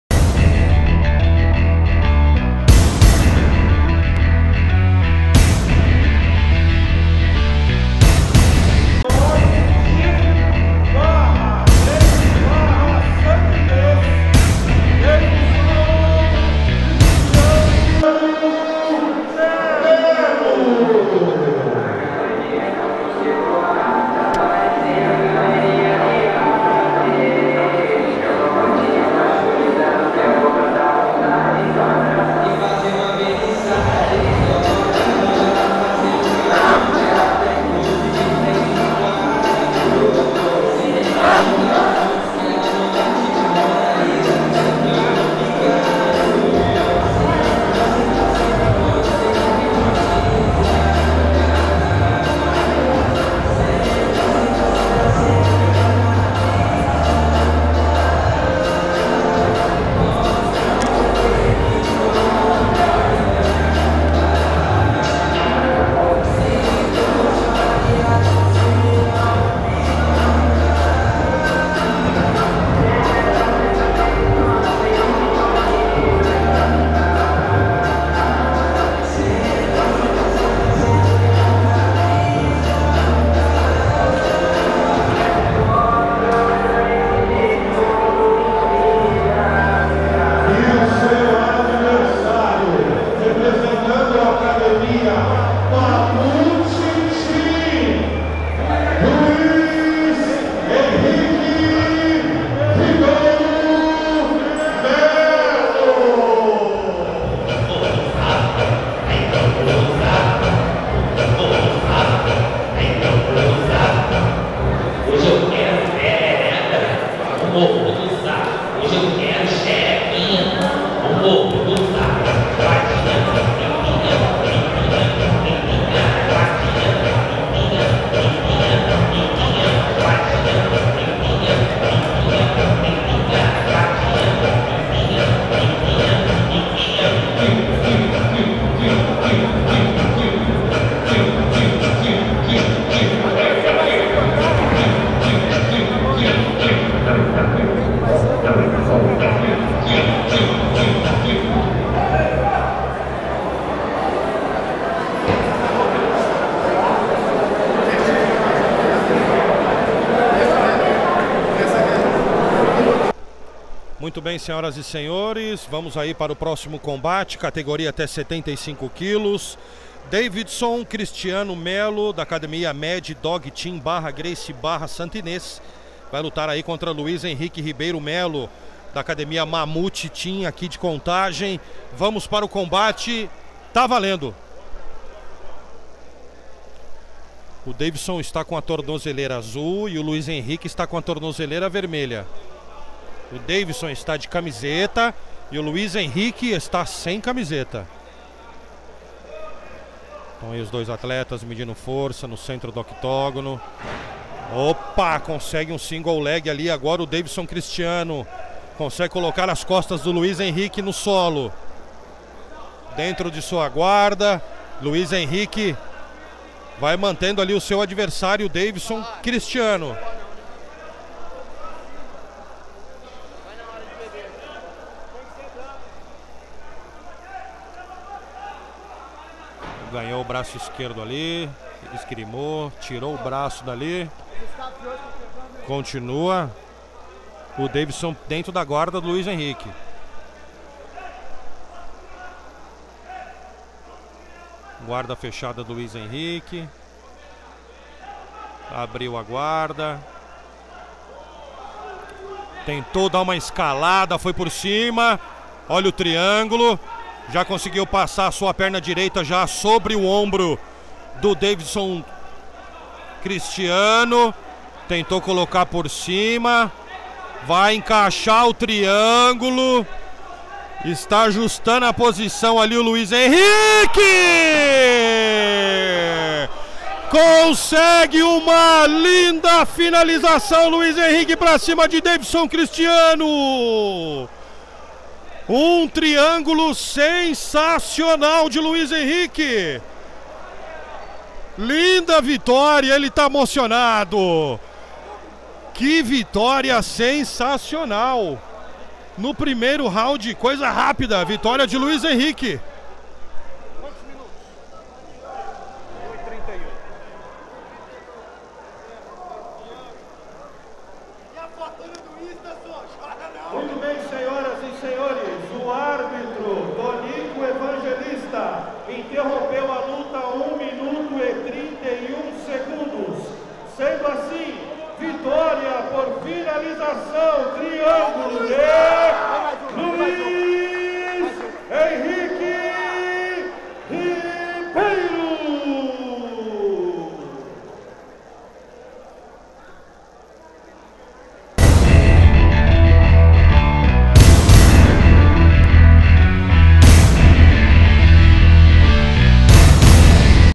Luiz vs Davison - X1 Kombat 8
Em combate de nogi até 75kg, Luiz Henrique (Mamute Team) venceu Davison Cristiano (Mad Dog Team/GB Santa Inês).